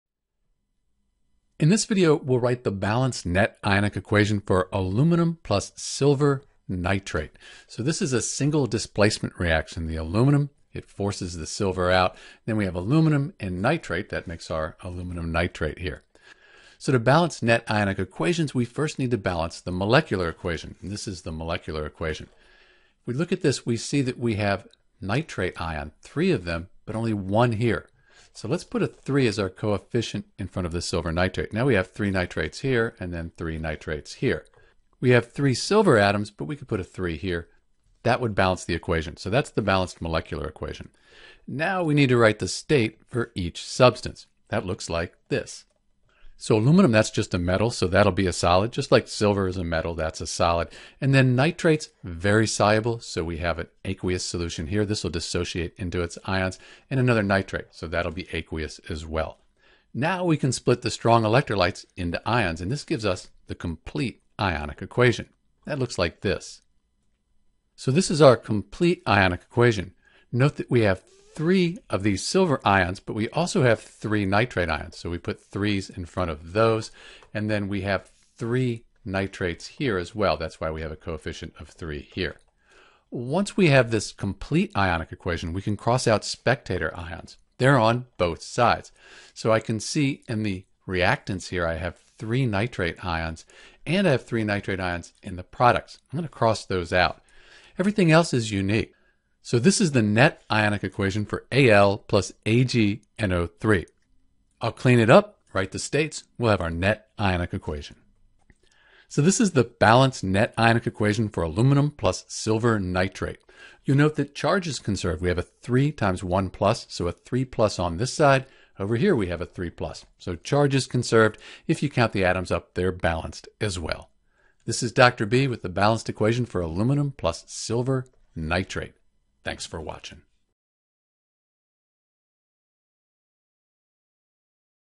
How to Write the Net Ionic Equation for Al + AgNO3 = Ag + Al(NO3)3
There are three main steps for writing the net ionic equation for Al + AgNO3 = Ag + Al(NO3)3 (Aluminum + Silver nitrate). First, we balance the molecular equation. Second, we write the states and break the soluble ionic compounds into their ions (these are the strong electrolytes with an (aq) after them). Finally, we cross out any spectator ions. These are the ions that appear on both sides of the ionic equation.

Important Skills

More Practice

General Steps:

1. Write the balanced molecular equation.
2. Write the state (s, l, g, aq) for each substance.
3. Split soluble compounds into ions (the complete ionic equation).
4. Cross out the spectator ions on both sides of complete ionic equation.
5. Write the remaining substances as the net ionic equation.